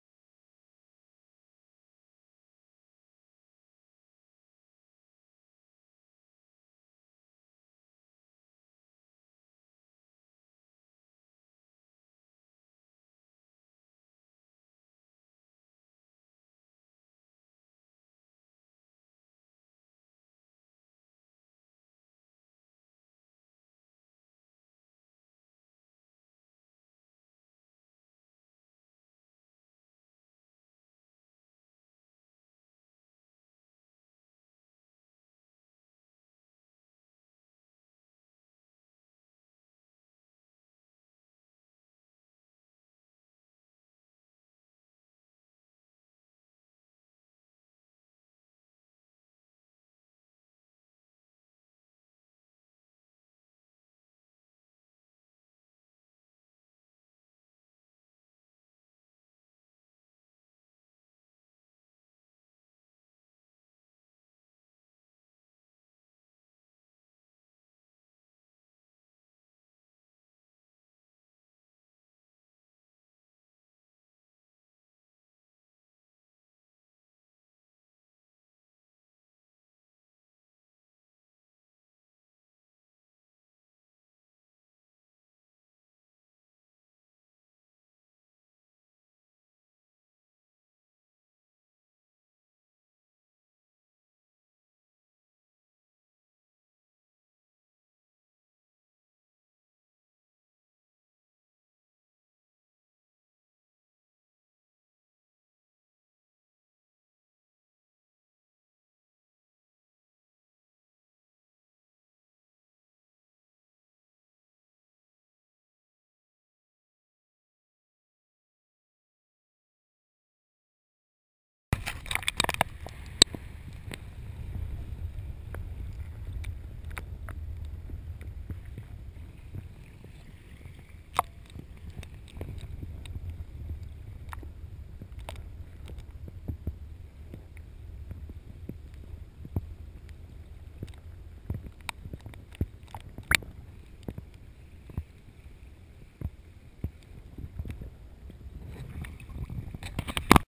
GoPro HERO 2 HD-Wild Water
Aufgenommen mit einer GoPro HERO 2 HD, mit 1025X80 Pixel.
Aufnahmeort war die Günz kurz bevor sie in die Donau bei Günzburg fließt.